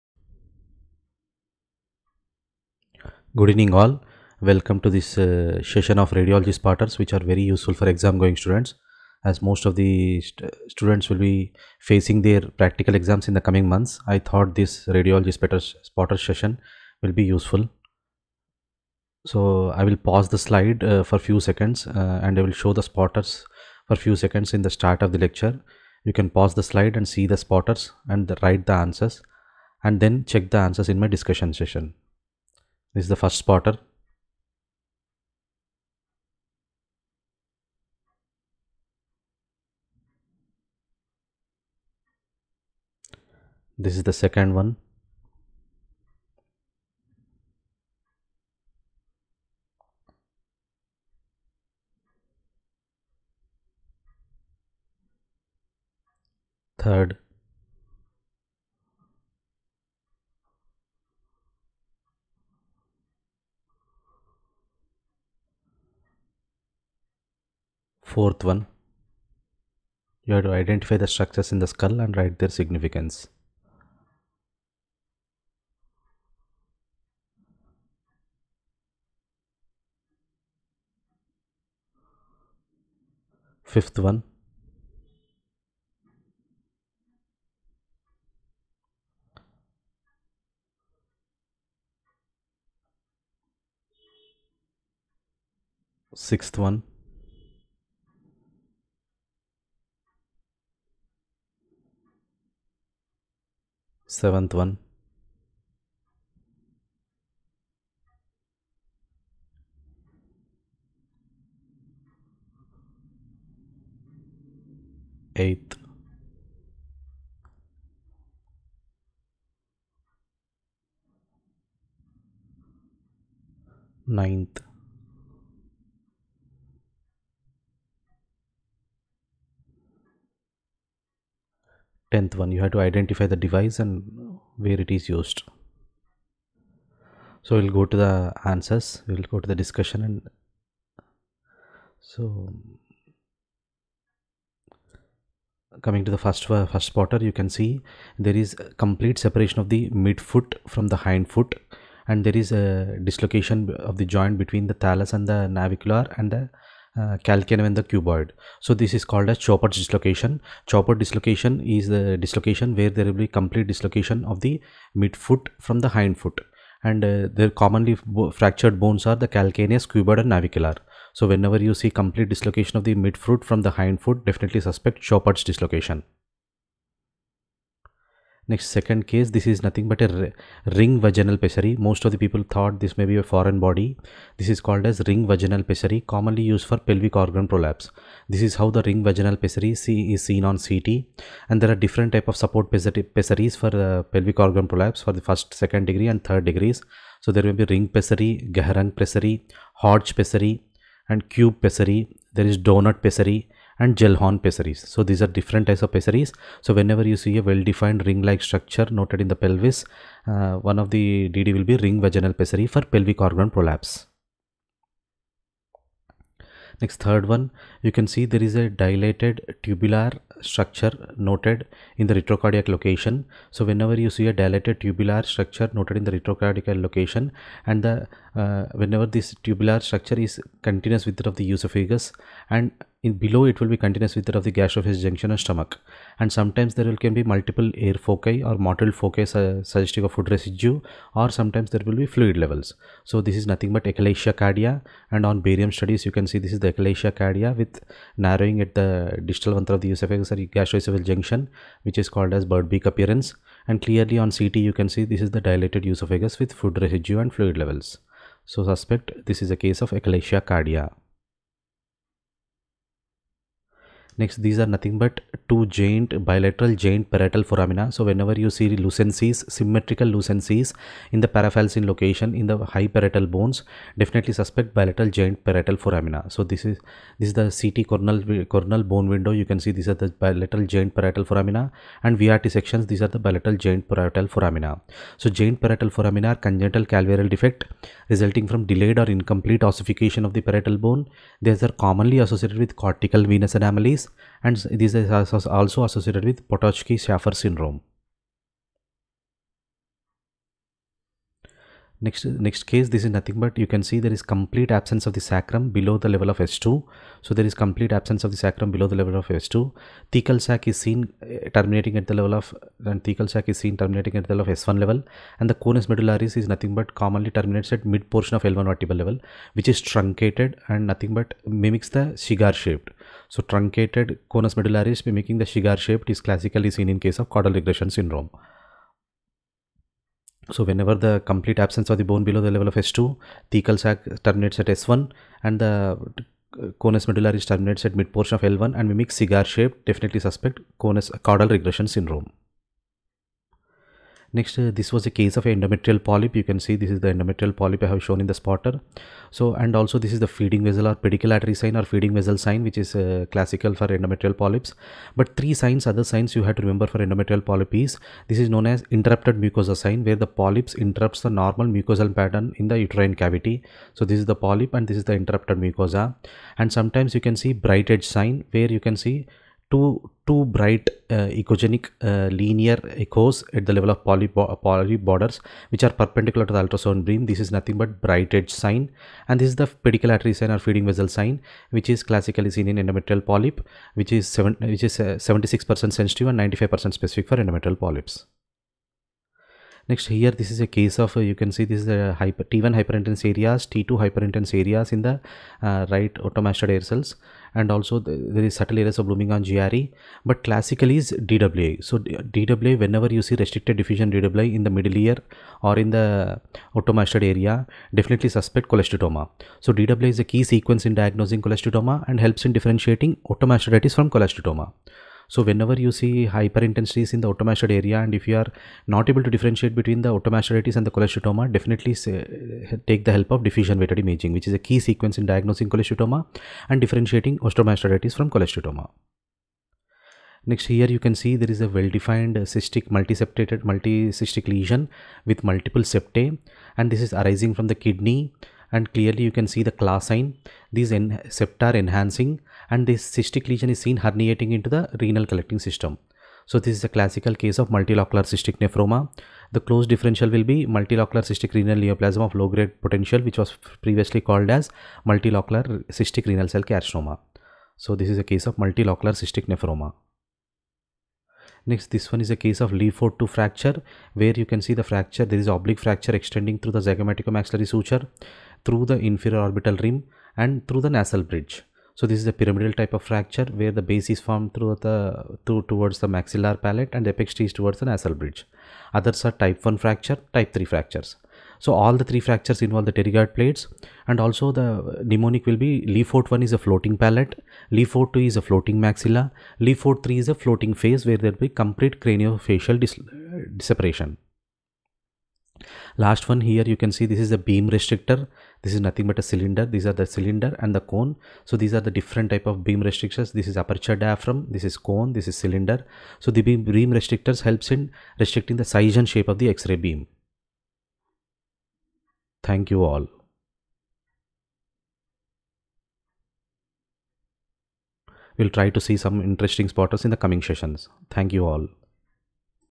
EXAM GOING STUDENTS RADIOLOGY SPOTTERS SET III
Important radiology spotters are discussed for exam going students and radiology residents